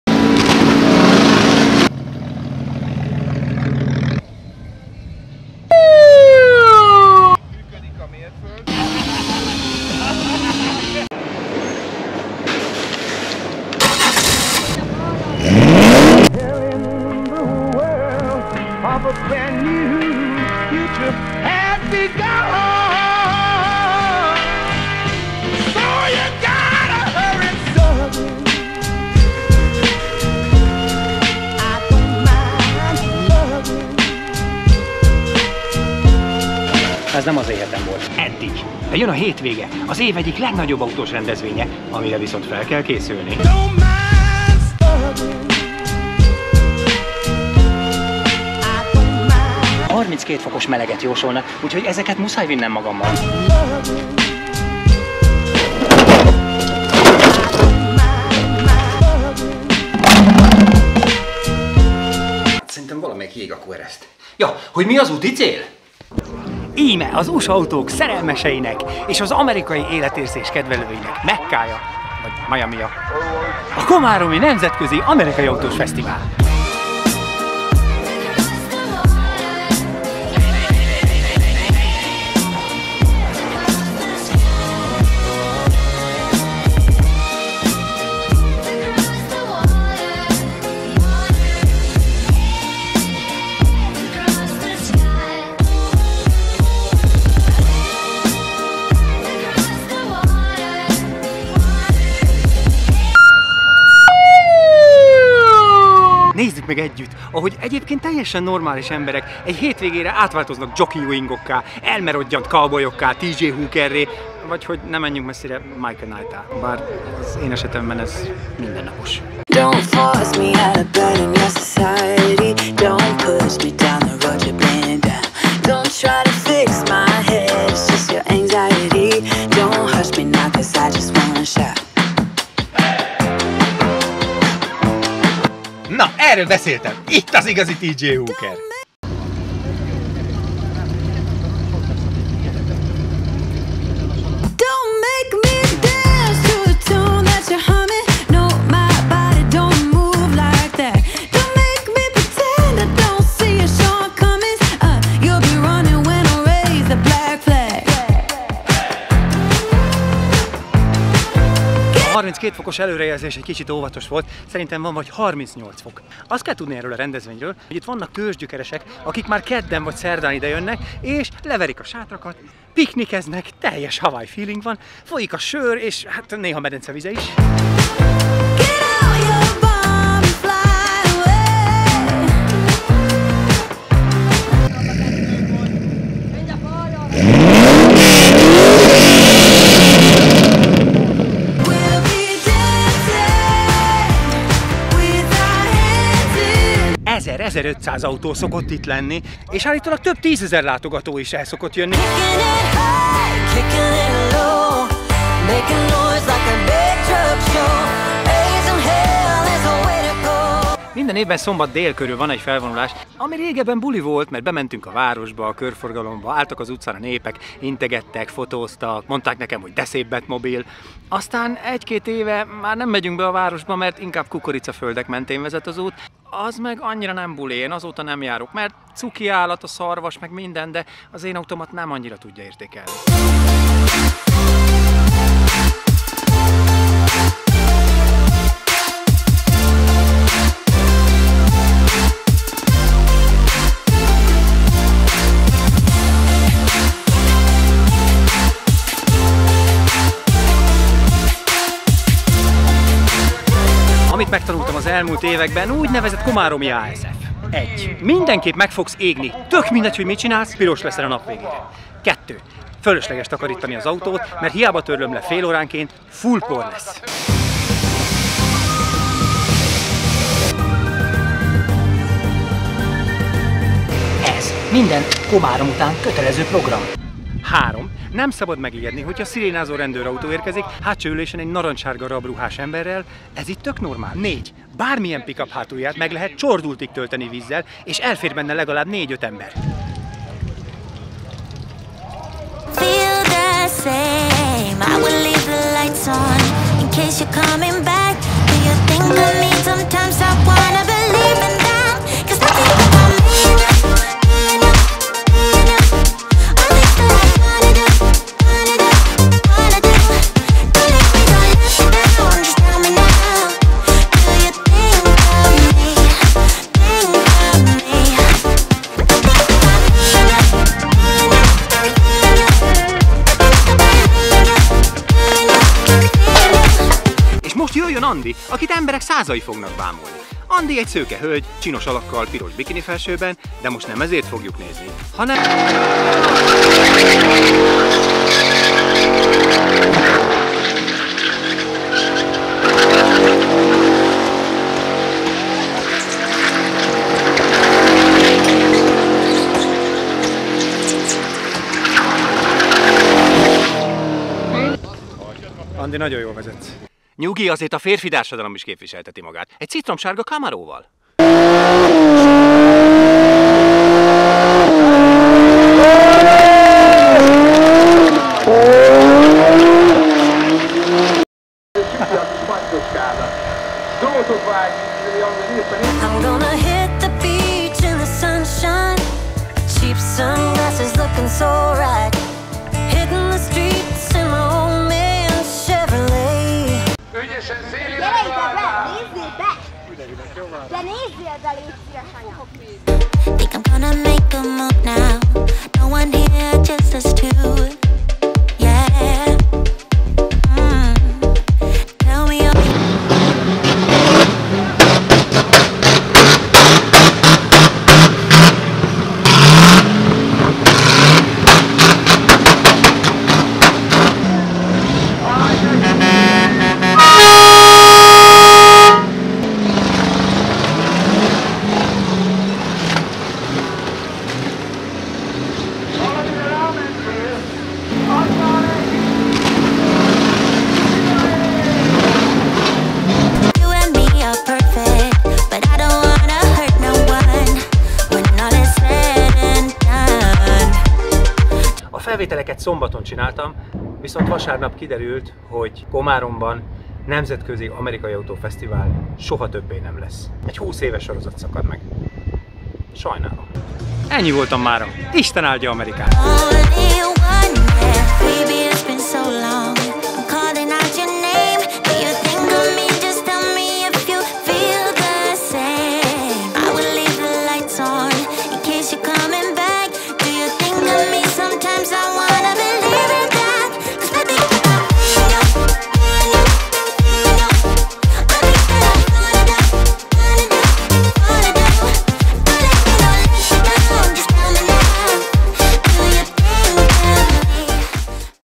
EZER AUTÓ/10.000 ember:UTOLSÓ Komárom...NAAF 2021
2021 NAAF-Komárom/ nemzetközi USA autó találkozó...ahogy én láttam.
❗Legyél csatornatag! ❗Előbb látod a videót és a TITKOS posztokat!